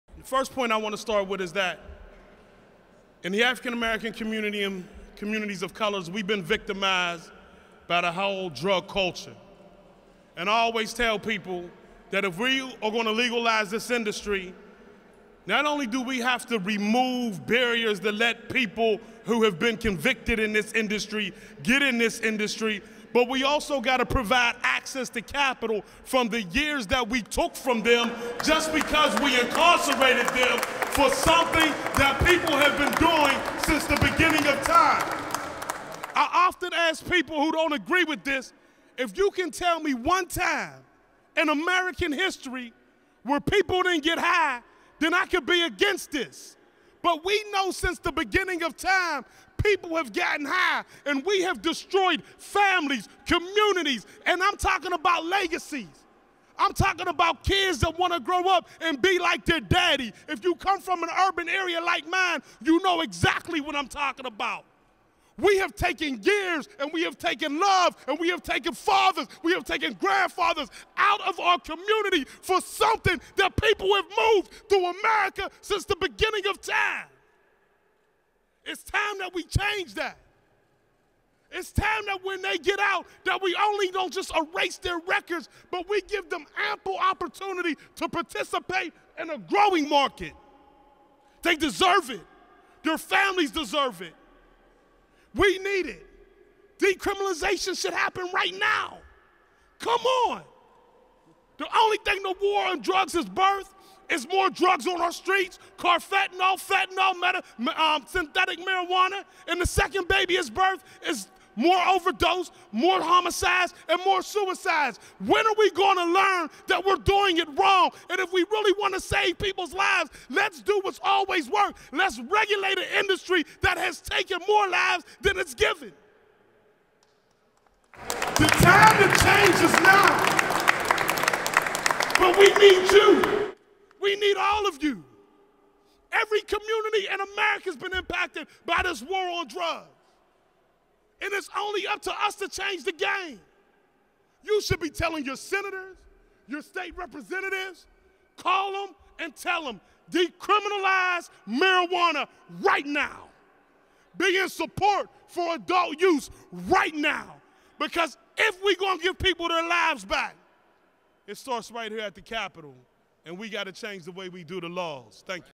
Gainey: Decriminalization of Marijuana
Pa state Rep. Ed Gainey is working to see adult use cannabis become legal in Pennsylvania. Gainey feels that decriminalization of marijuana should take effect in the Commonwealth.